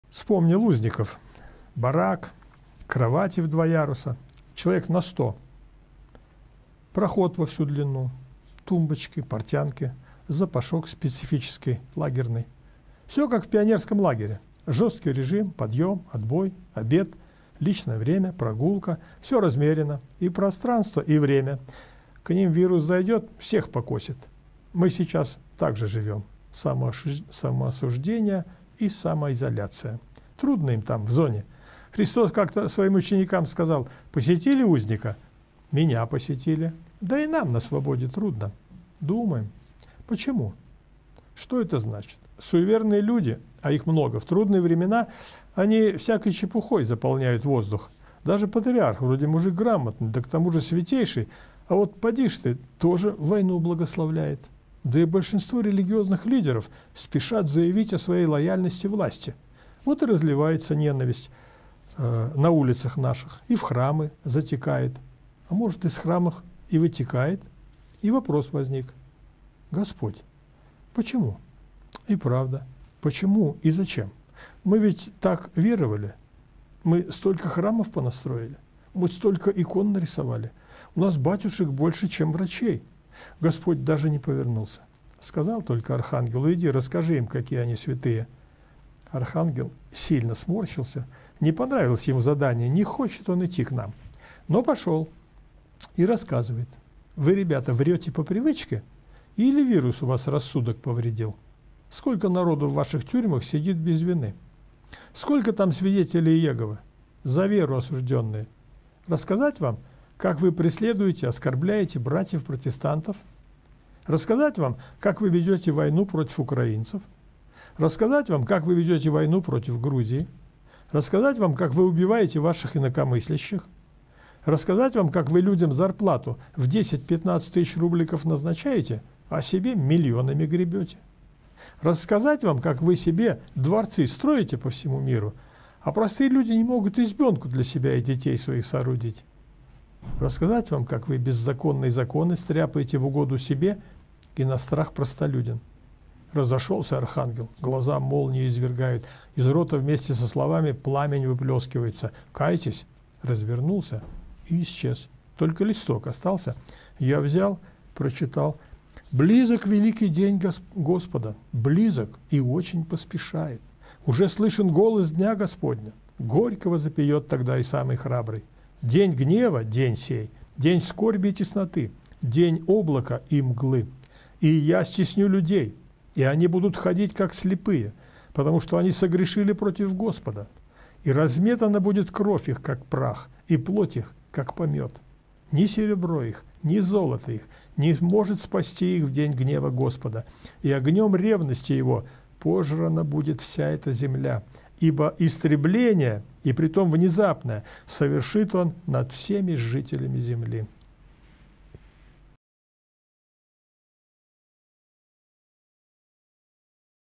Суеверные люди, а их много, в трудные времена | Чистый четверг | Юрий Сипко
Вспомнил узников. Барак. Кровати в два яруса. Человек на сто.
Проход во всю длину. Тумбочки. Портянки. Запашок специфический. Лагерный.
Всё как в пионерском лагере.
Жёсткий режим. Подъём. Отбой. Обед. Личное время.
Прогулка. Всё размерено. И пространство и время.
К ним вирус зайдёт, всех покосит.
Мы сейчас также живём. Самоосуждение на самоизоляцию.
Трудно им там, в зоне.
Христос как-то своим ученикам сказал: Посетили узника- Меня посетили.
Да и нам на свободе трудно.
Думаем. Почему? Что это значит? Суеверные люди, а их много, в трудные времена они всякой чепухой заполняют воздух. Даже Патриарх, вроде мужик грамотный, да к тому же святейший, а вот подишь ты, тоже войну благословляет.
Да и большинство религиозных лидеров спешат заявить о своей лояльности власти. Вот и разливается ненависть на улицах наших, и в храмы затекает. А может из храмов и вытекает?
И вопрос возникает. Господь, Почему?
И правда, почему и зачем? Мы ведь так веровали. Мы столько храмов понастроили.
Мы столько икон нарисовали. У нас батюшек больше чем врачей.
Господь даже не повернулся. Сказал только архангелу. Иди, расскажи им, какие они святые.
Архангел сильно сморщился. Не понравилось ему задание. Не хочет он идти к нам.
Но пошёл. И рассказывает.
Вы ребята, врёте по привычке, или вирус у вас рассудок повредил?
Сколько народу в ваших тюрьмах сидят без вины?
Сколько там Свидетелей Иеговы? За веру осуждённые.
Рассказать вам, как вы преследуете, оскорбляете братьев протестантов?
Рассказать вам, как вы ведёте войну против Украинцев?
Рассказать вам, как вы ведёте войну против Грузии?
Рассказать вам как вы убиваете ваших инакомыслящих?
Рассказать вам, как вы людям зарплату в десять-пятнадцать тыщ рубликов назначаете, а себе мильонами гребёте?
Рассказать вам, как вы себе дворцы строите по всему миру, и простые люди не могут избёнку для себя и детей своих соорудить. Рассказать вам, как вы беззаконные законы стряпаете в угоду себе и на страх простолюдин?
Разошёлся Архангел. Глаза молнии извергают. Из рота вместе со словами пламень выплёскивается.
Кайтесь! Развернулся и исчез.
Листок только остался.
Я взял. Прочитал.
Соф 1:14-15,17-18: "Близок великий день Господа, близок, и очень поспешает: уже слышен голос дня Господня; горько возопиет тогда и самый храбрый!  День гнева — день сей, день скорби и тесноты, день опустошения и разорения, день тьмы и мрака, день облака и мглы,  ... И Я стесню людей, и они будут ходить, как слепые, потому что они согрешили против Господа, и разметана будет кровь их, как прах, и плоть их — как помет.  Ни серебро их, ни золото их не может спасти их в день гнева Господа, и огнем ревности Его пожрана будет вся эта земля, ибо истребление, и притом внезапное, совершит Он над всеми жителями земли. "

Вы можете поддержать нас финансово:
YouTube канал ВЗГЛЯД С НЕБЕСНОЙ